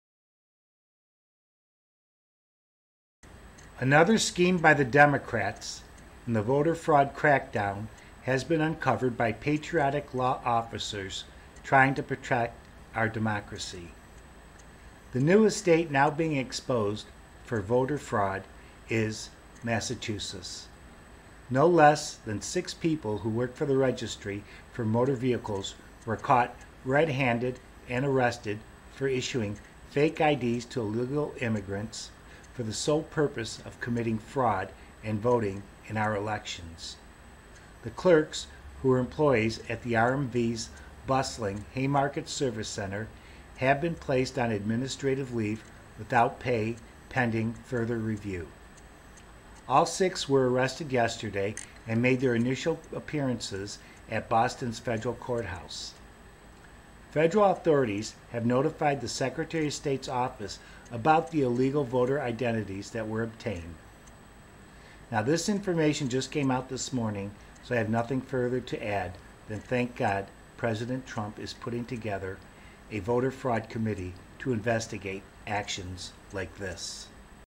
Massachusetts Registry voter fraud clerks arrested giving false IDs to illegal immigrants
Another scheme by the democrats in the voter fraud crackdown has been uncovered by patriotic law officers trying to protect our democracy.